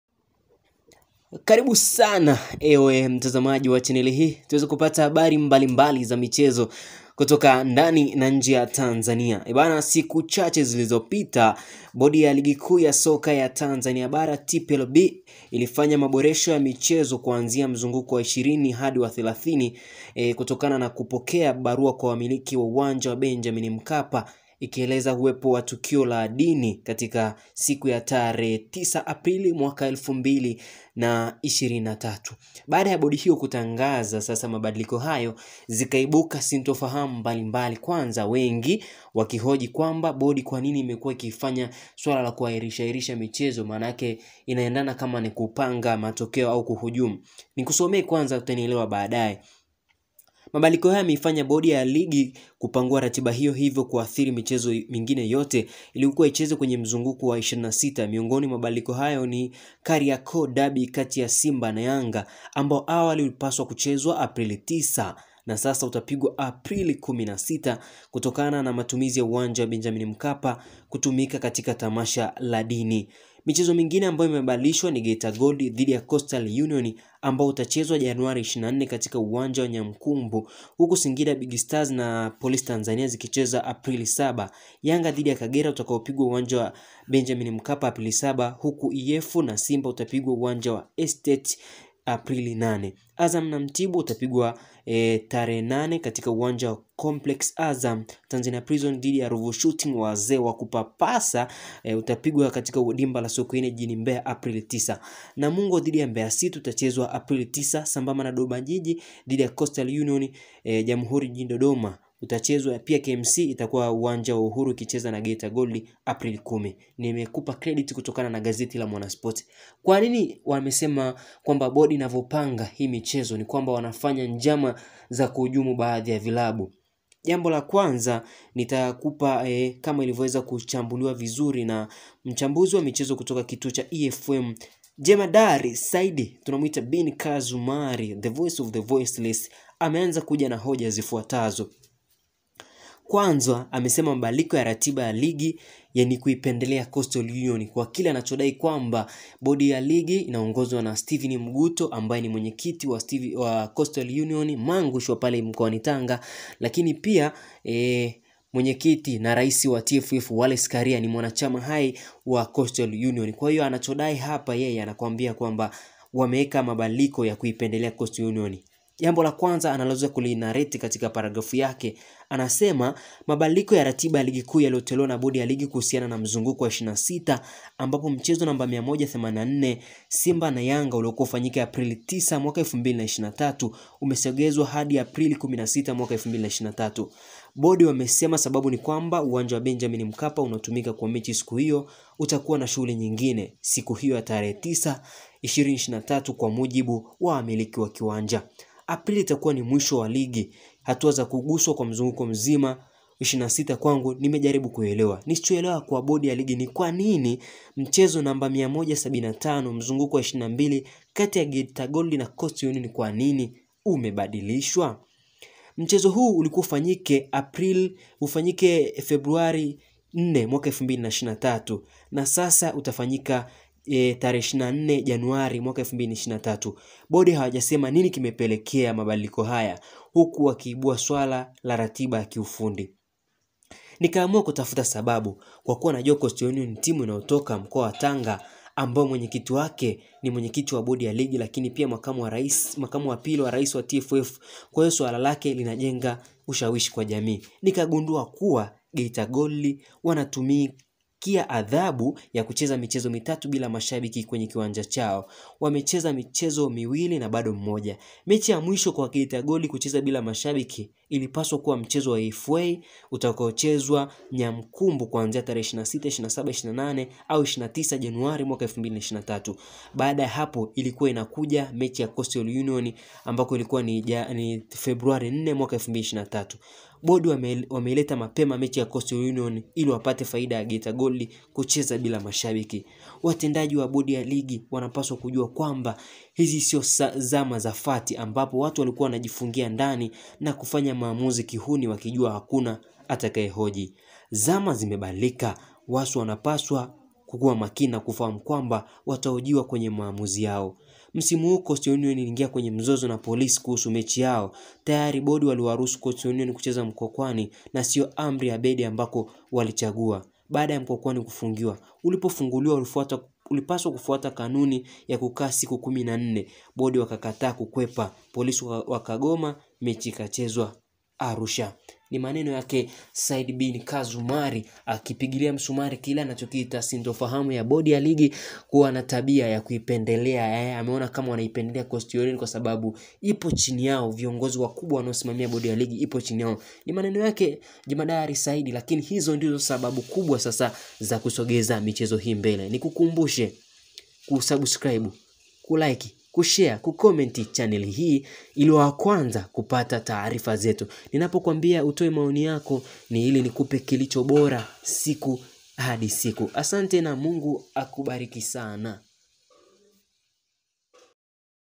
HEE! KUMBE CHANZO CHA KUHAIRISHA MECHI ZA YANGA, GEITA NI HIKI| Jemadari Atoa Mkasa mzima
MABADILIKO YA RATIBA, BODI YA LIGI KUU IMEIPENDELEA COASTAL UNION

Mabadiliko ya ratiba ya Ligi Kuu yaliyotolewa na Bodi ya Ligi kuhusiana na mzunguko wa 26 ambapo mchezo namba 184 Simba SC Vs Yanga SC uliokuwa ufanyike tarehe 9 Aprili, 2023 umesogezwa hadi tarehe 16 Aprili, 2023.

Bodi wamesema sababu ni kwamba Uwanja wa Benjamin Mkapa unaotumika kwa mechi hiyo utakuwa na shughuli nyingine siku ya tarehe 9 Aprili, 2023 kwa mujibu wa wamiliki wa kiwanja.

Aprili itakuwa ni mwisho mwisho mwa ligi, hatua ya kuguswa mzunguko mzima wa 26 kwangu nimejaribu kuielewa.

Nisichoelewa kwa Bodi ya Ligi ni kwanini mchezo namba 175 mzunguko wa 22 kati ya Geita Gold na Coastal Union umebadilishwa?

Mchezo huu ulikuwa ufanyike tarehe 4 February, 2023 na sasa utafanyika tarehe 24 January, 2023. Bodi hawajasema nini kimepelekea mabadiliko haya huku wakijua ratiba ni suala la kiufundi.

Nikaamua kutafuta sababu kwakuwa najua Coastal Union ni timu inayotoka mkoa wa Tanga ambayo Mwenyekiti wake ni Mwenyekiti wa Bodi ya Ligi lakini pia Makamu wa Pili wa Rais wa TFF, kwahiyo suala lake linajenga ushawishi kwa jamii.

Nikagundua Geita Gold wanatumikia adhabu ya kucheza mechi 3 bila mashabiki kwenye kiwanja chao na wameshacheza mechi 2 bado 1.

Mechi ya mwisho kwa Geita Gold kucheza bila mashabiki ilipaswa kuwa kwenye mchezo wa FA uliopangwa kufanyika Nyankumbu kuanzia tarehe 26, 27,28 na 29 January, 2023.

Baada ya hapo inakuja mechi ya Coastal Union tarehe 4 February, 2023.

Bodi wameileta mapema mechi ya Coastal Union ili wapate faida ya Geita Gold kucheza bila mashabiki.

Watendaji wa Bodi ya Ligi wanapswa kujua kwamba hizi sio zama za FAT, ambapo watu walikuwa wanajifungia ndani na kufanya maamuzi kihuni wakijua hakuna atakaehoji.

Zama zimebadilika  watu wanapaswa kuwa makini na kufahamu kwamba watahojiwa kwenye maamuzi yao.

Msimu huu Coastal Union iliingia kwenye mzozo na Polisi kuhusu mechi yao na tayari Bodi waliwaruhusu Coastal kucheza Mkwakwani na sio Amri  Abeid ambako walichagua baada ya Mkwakwani kufungiwa, ulipofunguliwa ulipaswa kufuata kanuni ya kukaa siku 14, Bodi wakataka kukwepa. Polisi wakagoma, mechi ikachezwa Arusha.